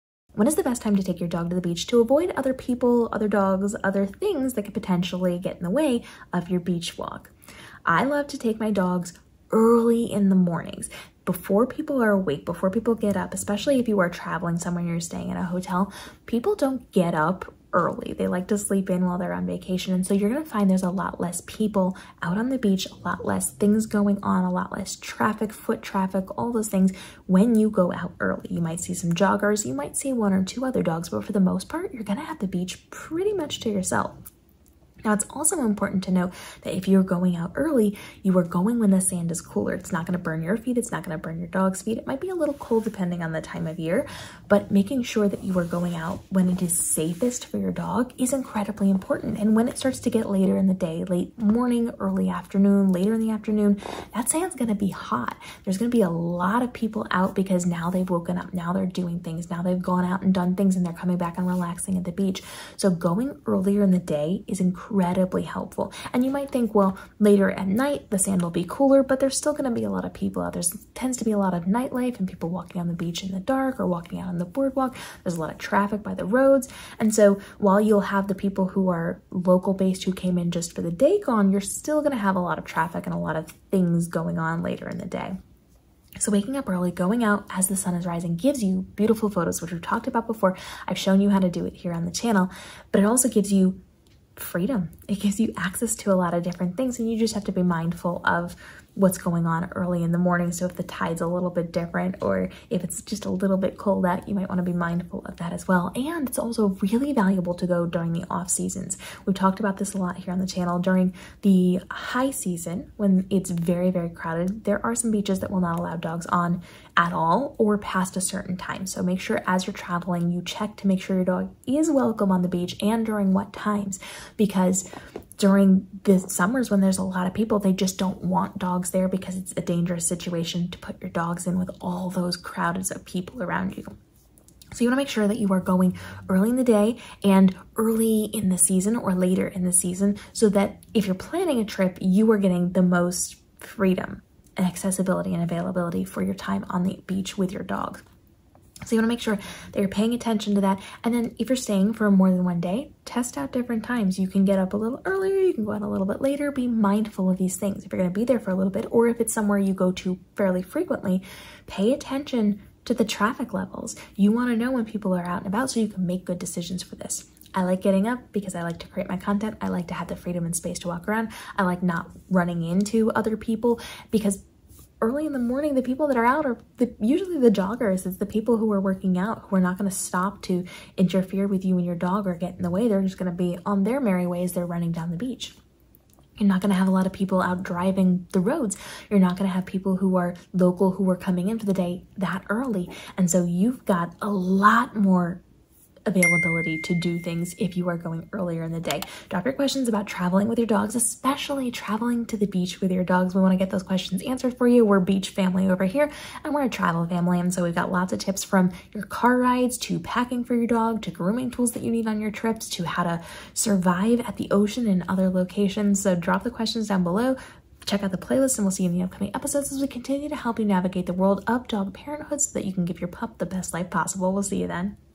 The Best Time Take Your Dog To The Beach To Avoid Other Dogs
When is the best time to take your dog to the beach to avoid other people, dogs, traffic, etc? Here's what to know!